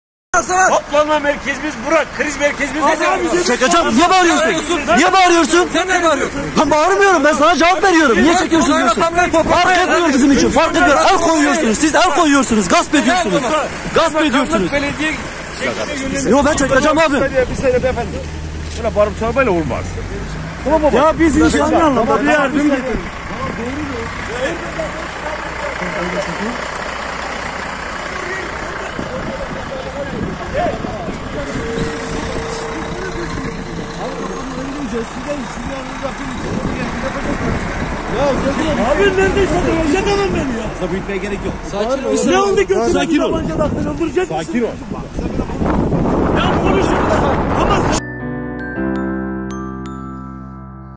POLİS MELETÎ’DE GİDEN YARDIM TIR’INA EL KOYDU
Halkların Demokratik Partisi (HDP) Mêrdîn İl Örgütü tarafından depremin yaşandığı Meletî’nin Wêranşar (Doğanşehir) ilçesinde gönderilen gıda ve temel yaşam malzemelerinin olduğu TIR ilçe girişinde TEM polisleri tarafından durdurularak, yardımın ulaşmasını engelledi. Aç, susuz ve açıkta olan insanlara gönderilen malzeme yüklü TIR’a el koyan polisler, tüm itirazlara rağmen geri vermedi. HDP İl Örgütü tarafından içinde yaşam malzemelerinin bulunduğu TIR’a el koyan polis, AFAD dışında yardım yapılmasına izin vermeyeceklerini söyledi.